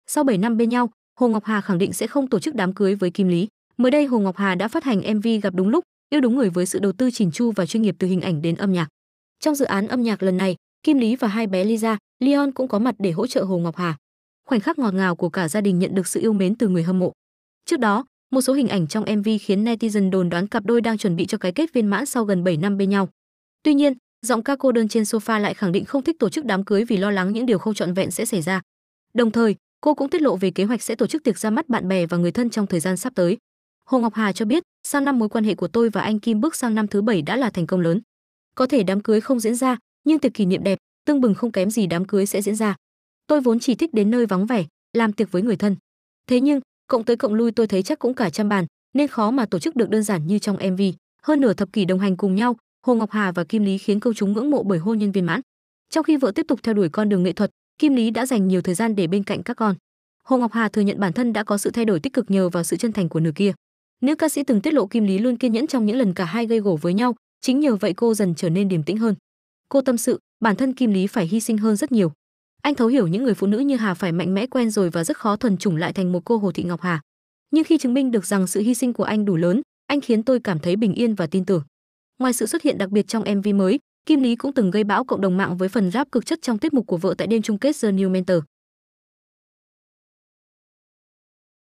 Hồ Ngọc Hà chia sẻ lý do không tổ chức đám cưới với Kim Lý
Hồ Ngọc Hà khẳng định không thích tổ chức đám cưới vì lo lắng những điều không trọn vẹn sẽ xảy ra.

Nguồn hình ảnh: Hồ Ngọc Hà, Yeah 1